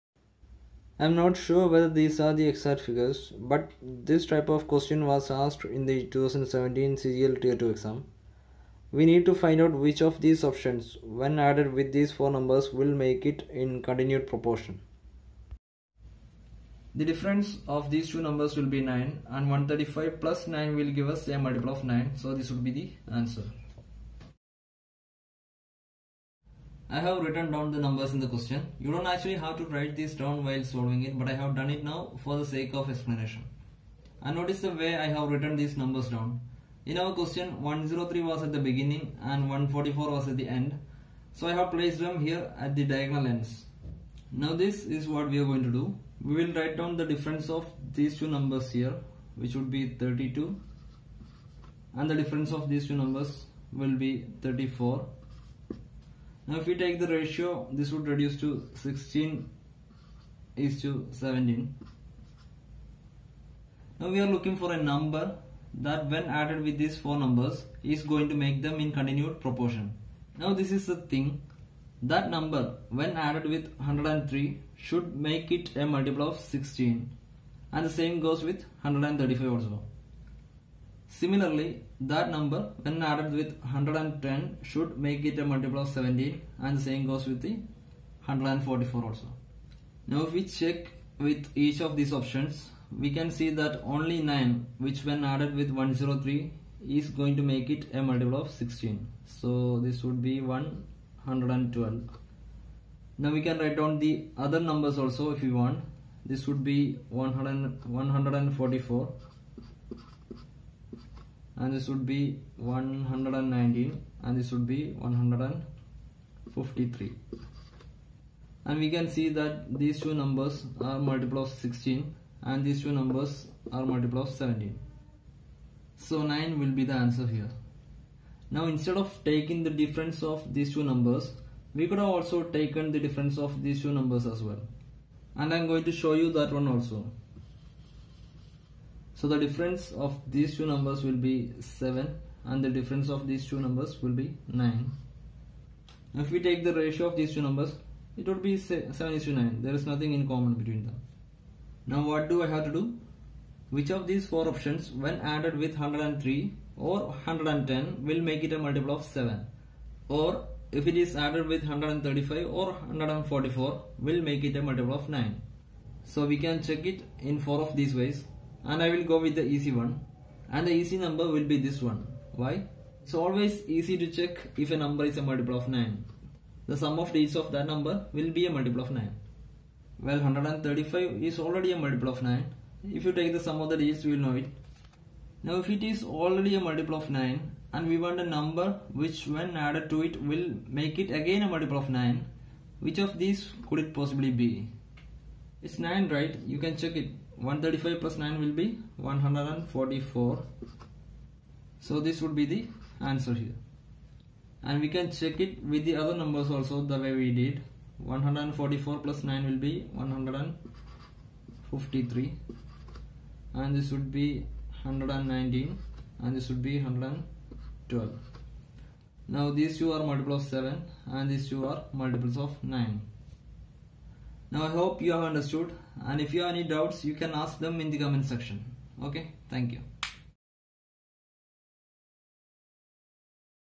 Shortcut : What number should be added to get continued proportion ?
Shortcut for the question asked in CGL Tier 2 exam.